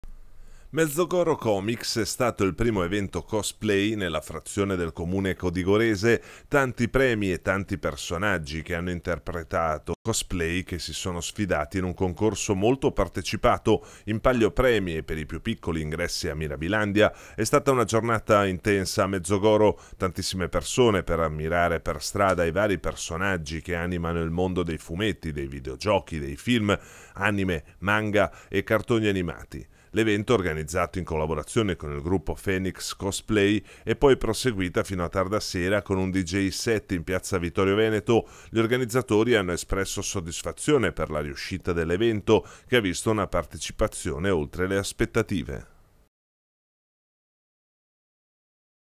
Mezzogoro Comics: successo per la prima edizione
Successo per la prima edizione del Mezzogoro Comics, la manifestazione che domenica 23 giugno che ha portato nella frazione codigorese tanti giovani e non solo, appassionati di fumetti, film e videogiochi.